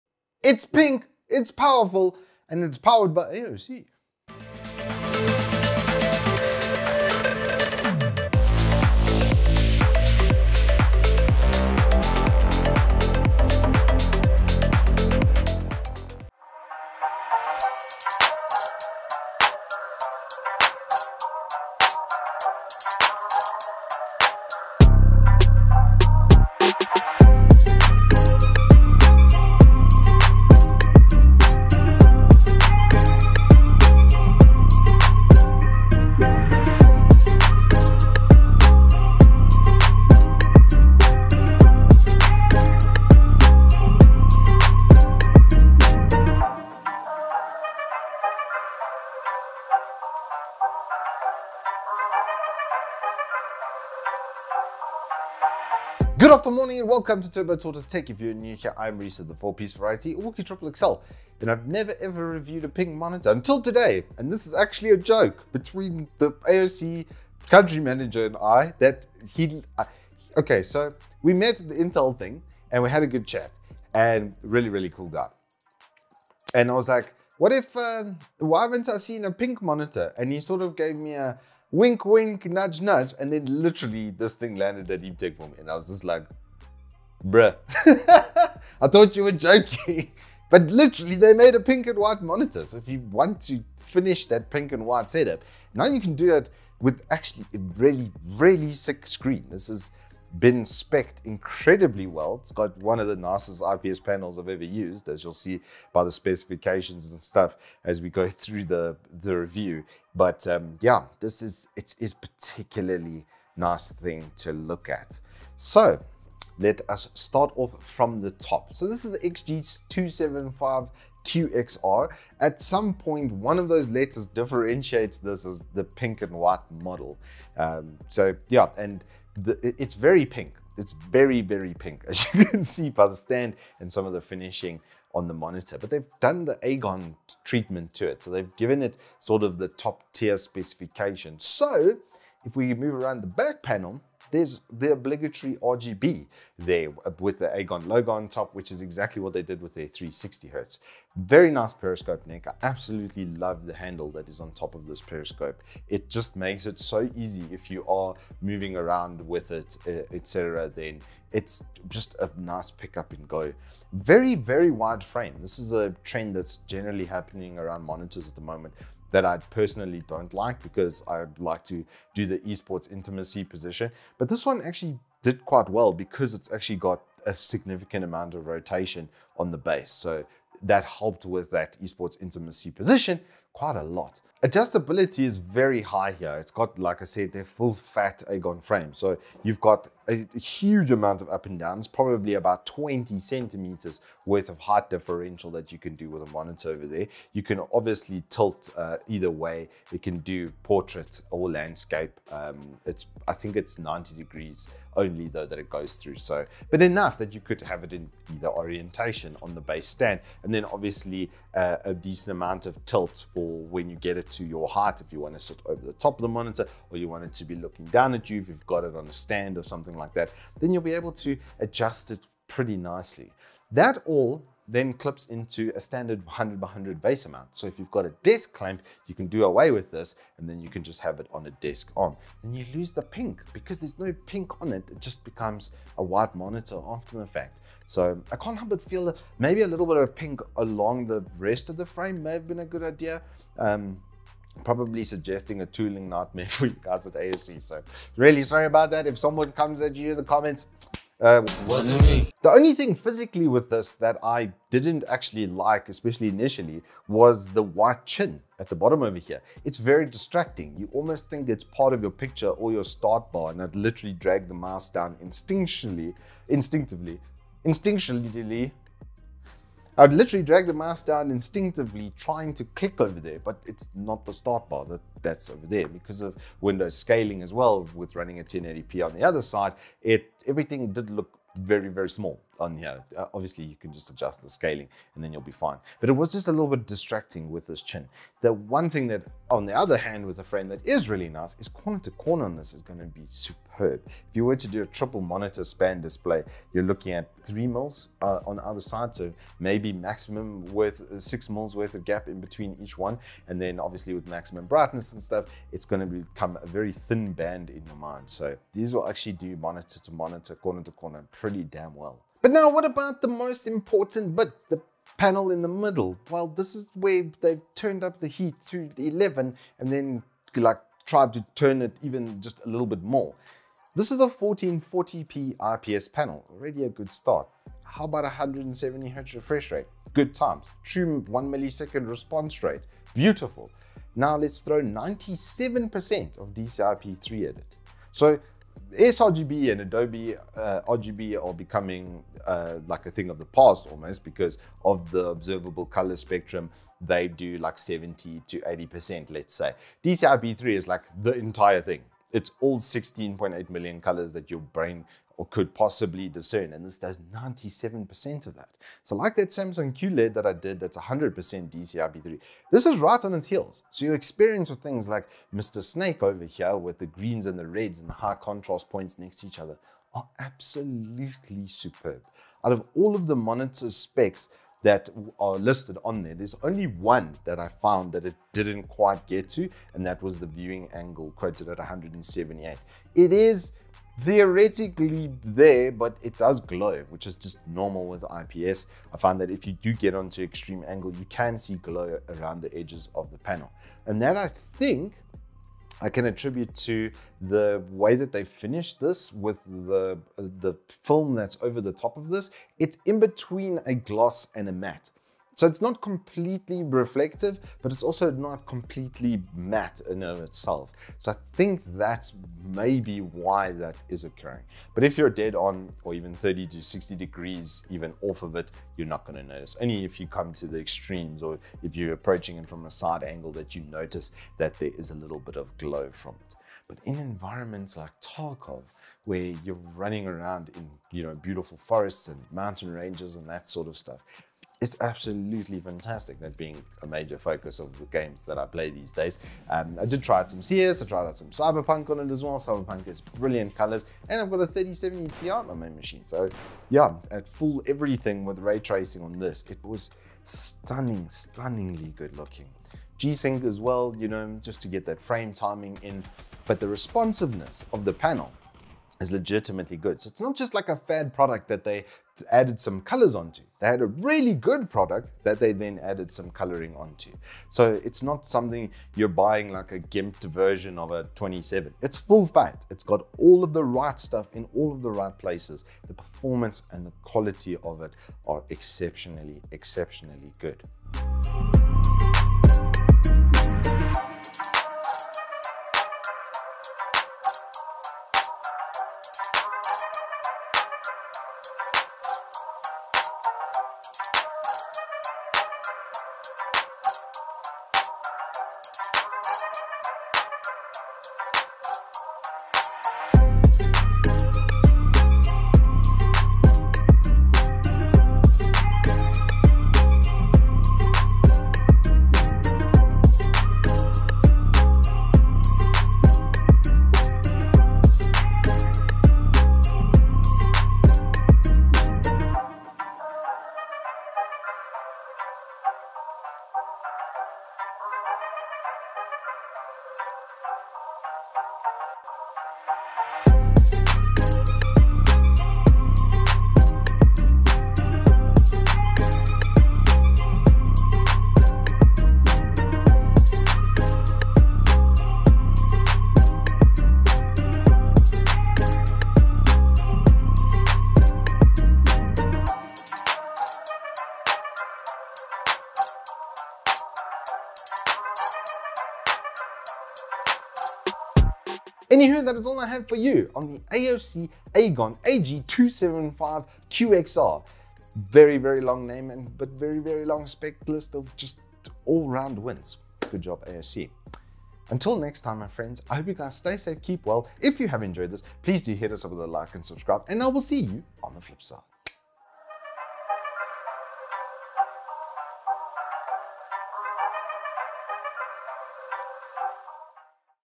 AOC Agon AG275QXR Review - A pink monitor?? MEME or BATTLE MAIDEN?
Gooooooooood after morning and welcome to another Turbo Tortoise Tech review!

This new monitor from AOC struck the wild Wooks outta nowhere like an RKO at the Super Slam. What was even more surprising was the sheer quality of the panel. It wasn't long before our Turbo Tortoise Techer was off to google in search of information on this exceptional panel... As it turns out his suspicions were correct and this thing is no slouch. Join the Wooks for the full review right here on Evetech Tube!

Agree or disagree? We would love to hear your thoughts on it in the comments down below!

For more awesome gaming and tech content as well as spicy giveaways follow us on the socials below:

0:00 Intro
0:17 Hottie with a body
0:55 Build Up
1:59 Monitor Build and Stand
5:12 Panel and Performance
8:01 OSD Control Panel Demo
9:21 Outro